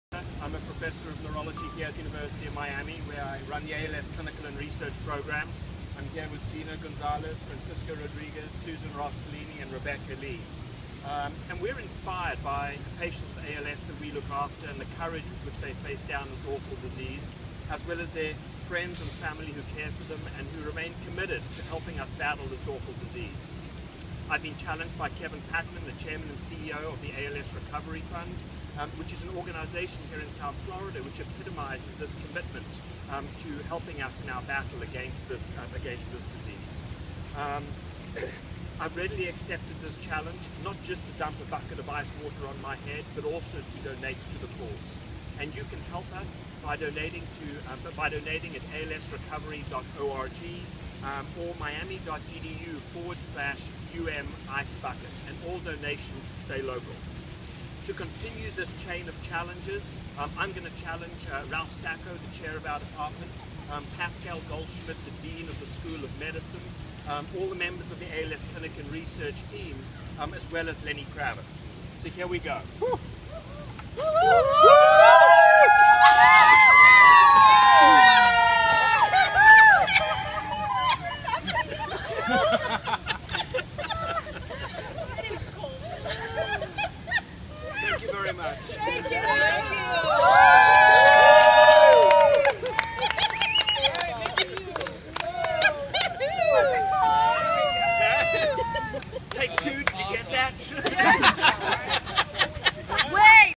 Michael Benatar, M.D., Ph.D. Accepts ALS Ice Bucket Challenge
Dr. Benatar has a well-developed research program specializing in Amyotrophic Lateral Sclerosis (ALS) and Myasthenia Gravis (MG). Please see the link below for information about some of his current and past research studies as well as publications